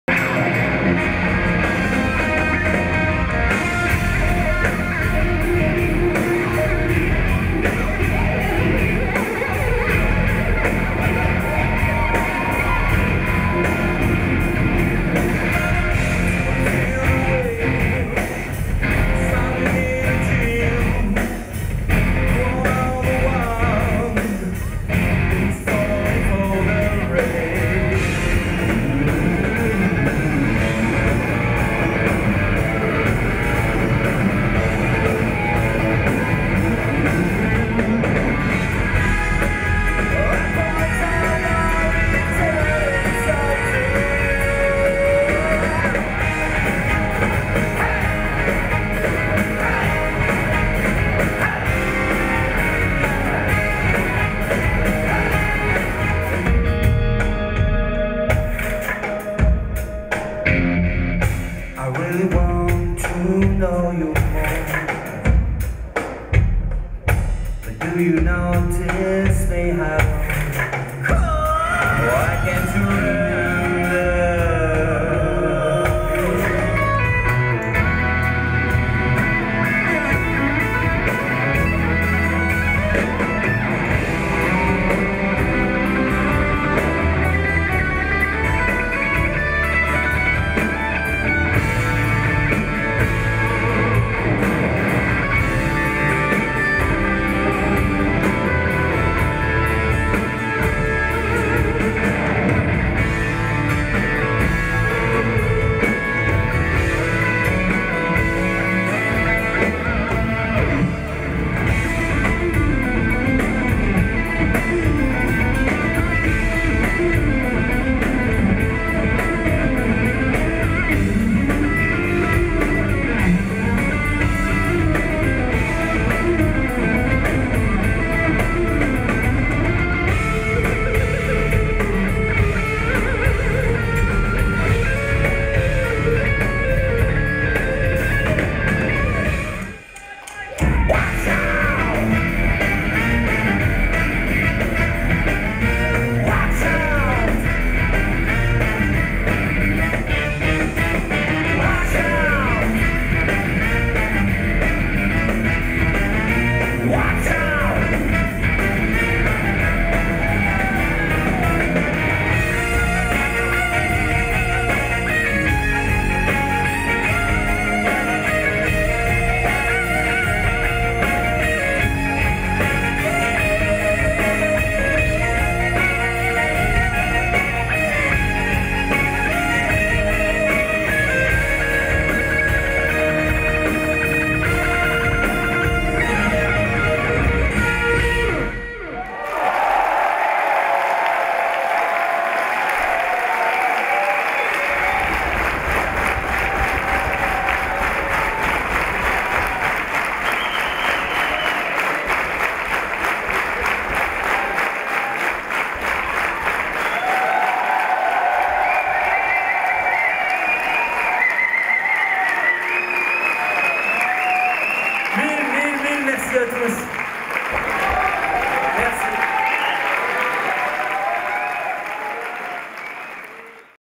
Pat O'May Live
Le DVD sortira en Septembre, mais cette bande annonce vous fera patienter comme il se doit !
Réalisation et montage DelfikProd.